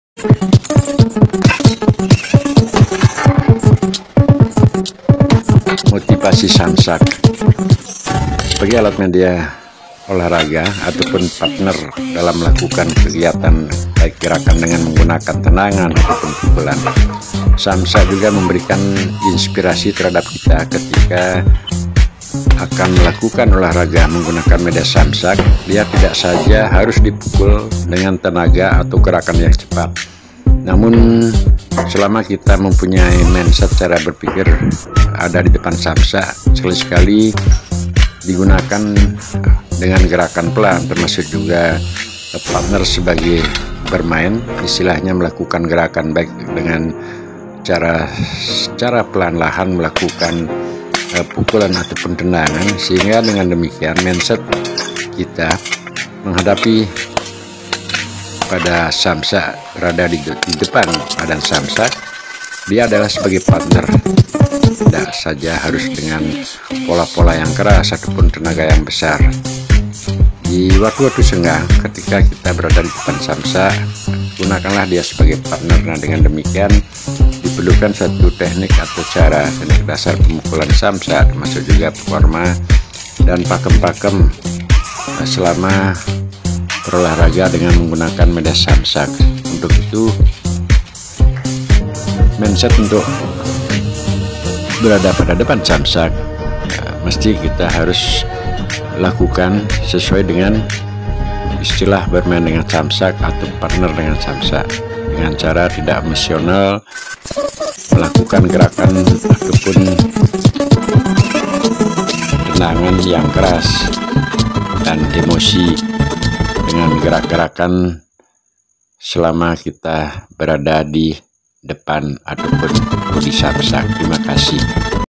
Motivasi samsak @ibanommustika4128
motivasi samsak sebagai partner olahraga, selama kita punya cara penggunaan samsak, maka dari itu untuk dapatnya Kita memungsikan samsak sebagai partner atau teman bermain di perlukan teknik dasar pukul dan tendang samsak.

pada channel Saya ini  ..,... dengan konsep berbagi ada banyak cara untuk penggunaan samsak.

silahkan teman kerabat melihatnya, untuk menambah pengalaman dalam memotivasi diri. 🙏 tkasih.